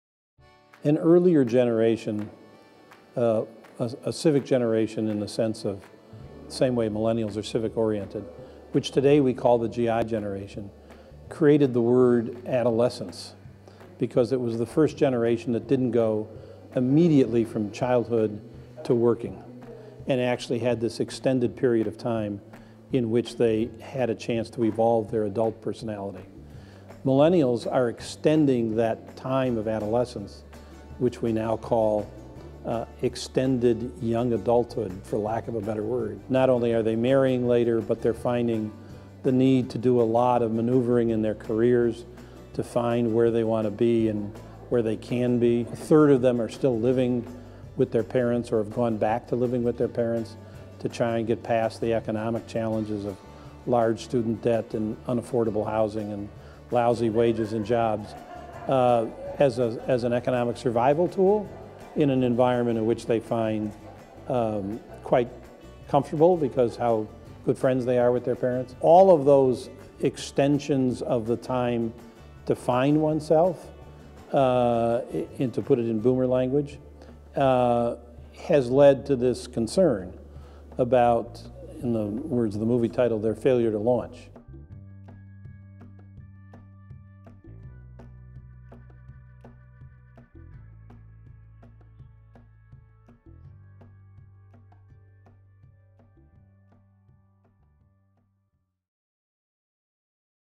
6 things marketers should know about extended young adulthood, part 1
What is extended young adulthood? It's a new phrase that describes the trend of Millennials prolonging the time between adolescence and full adulthood. It's a reality that wouldn't matter to the marketing community except that it has started to shift the normal pattern of purchasing behavior.

Millennial expert Morley Winograd speaks with iMedia about the signs of extended young adulthood and how Millennials are changing normal demographic behavioral beliefs.